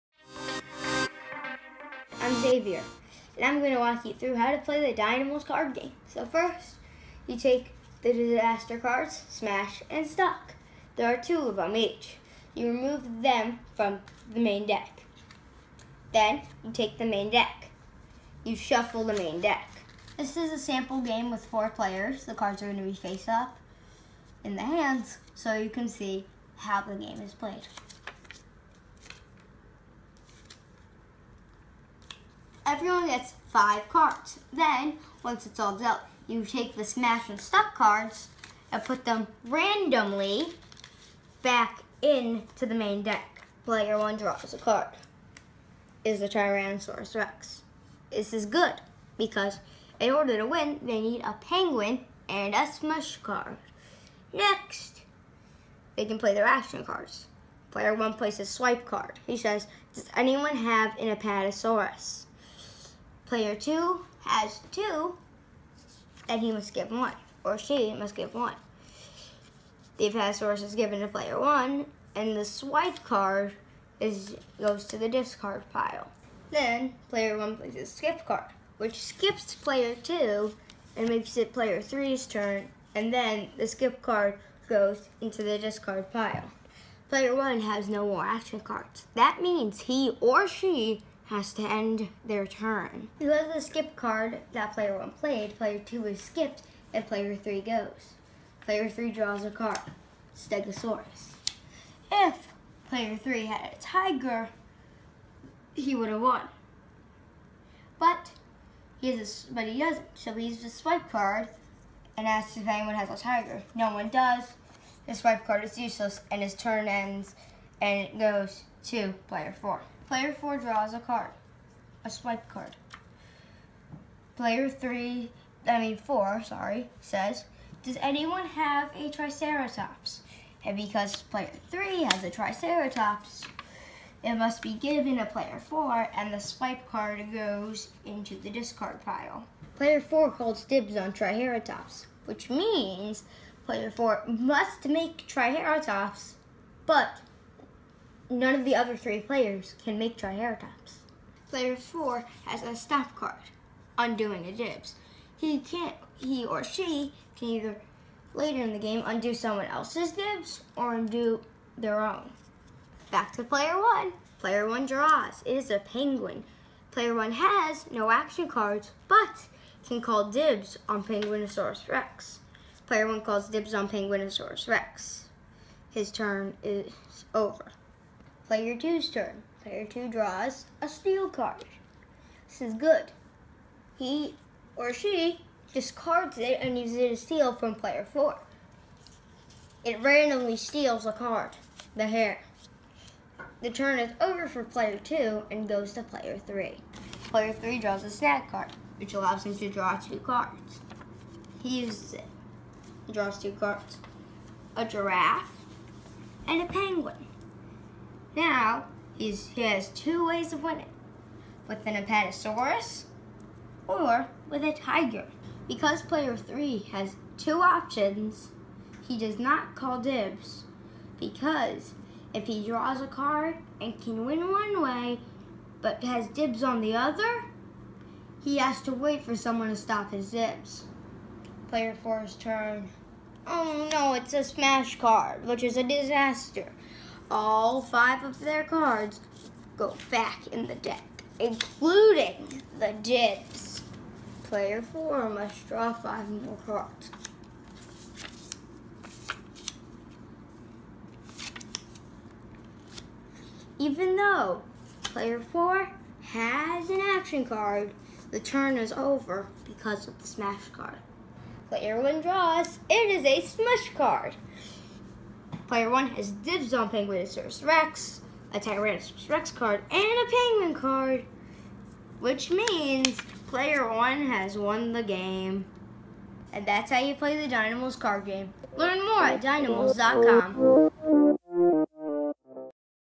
Dinomals sample game
Zavier walks through a sample hand. Launching Sept 13 on Kickstarter!